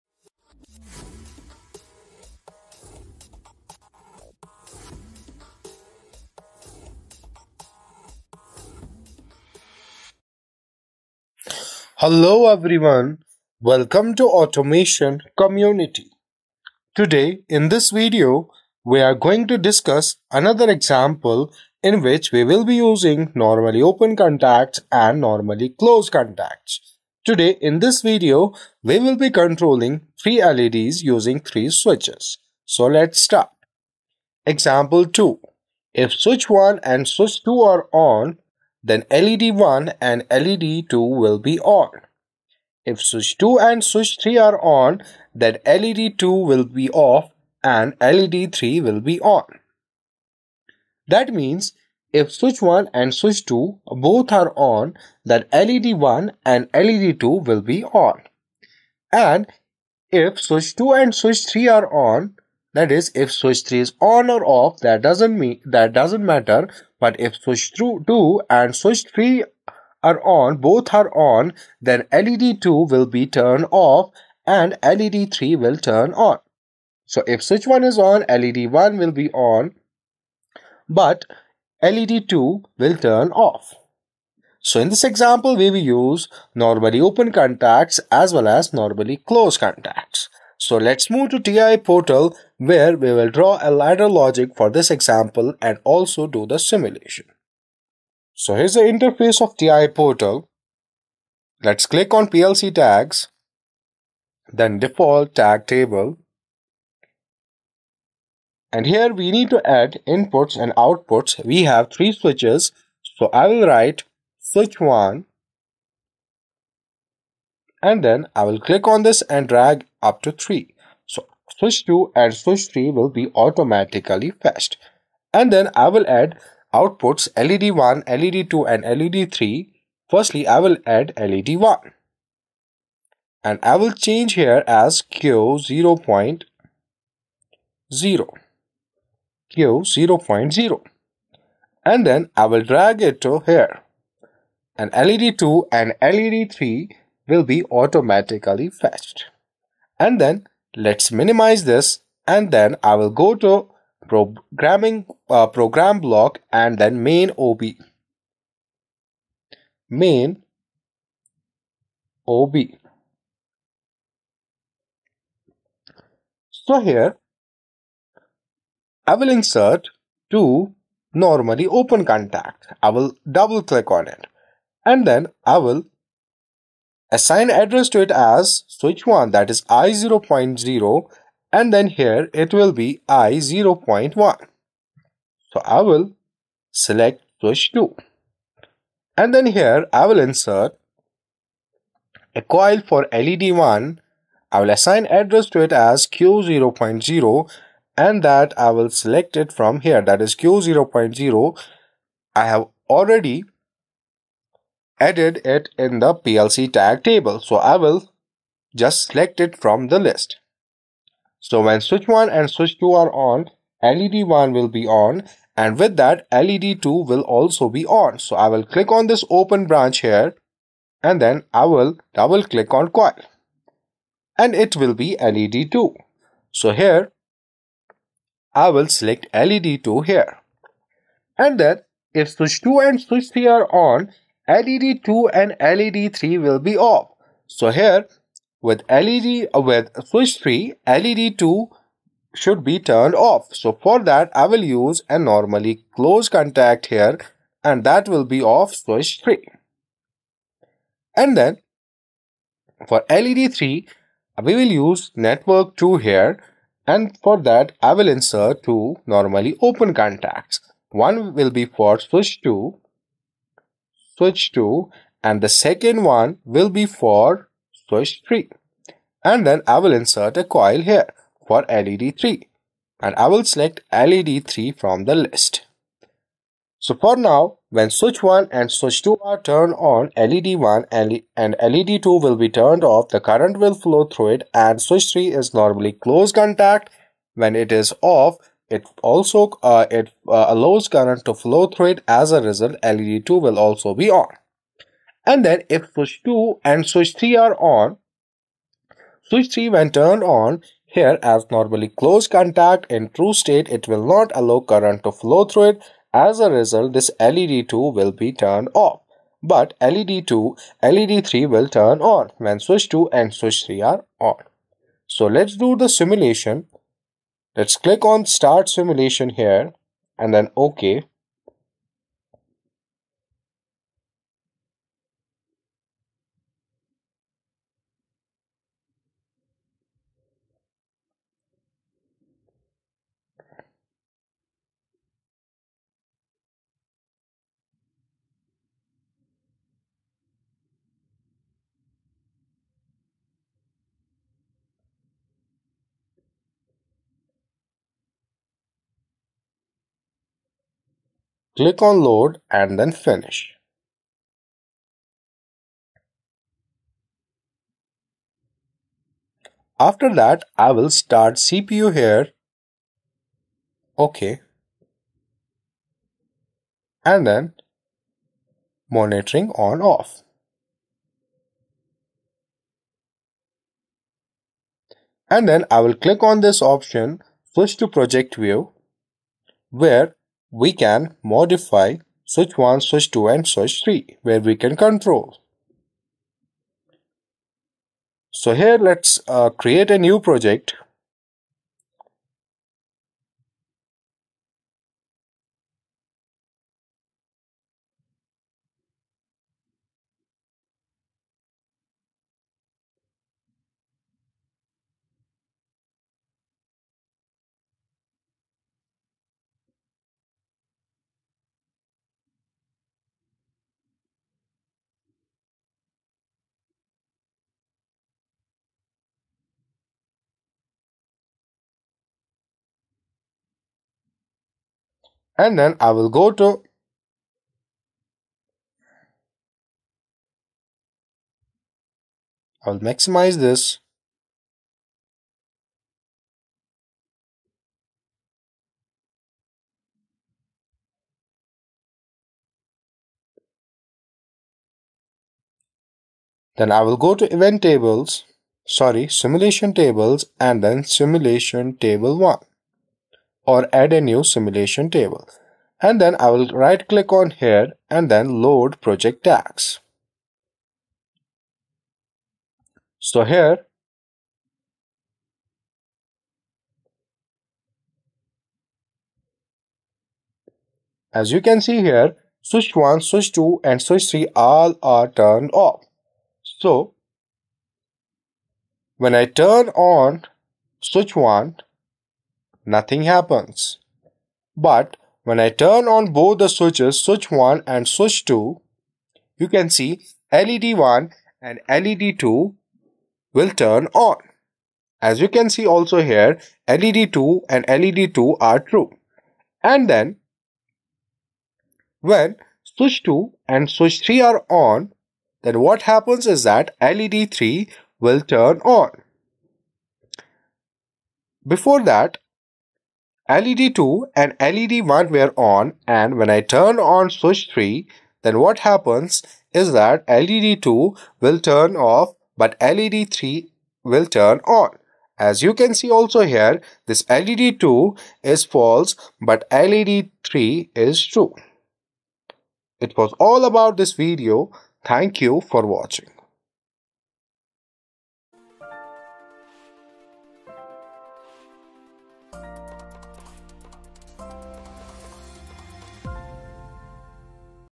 Siemens TIA Portal Basics: Easy PLC Examples for Beginners
Start your PLC programming journey with Siemens TIA Portal basics, featuring easy-to-follow examples perfect for beginners.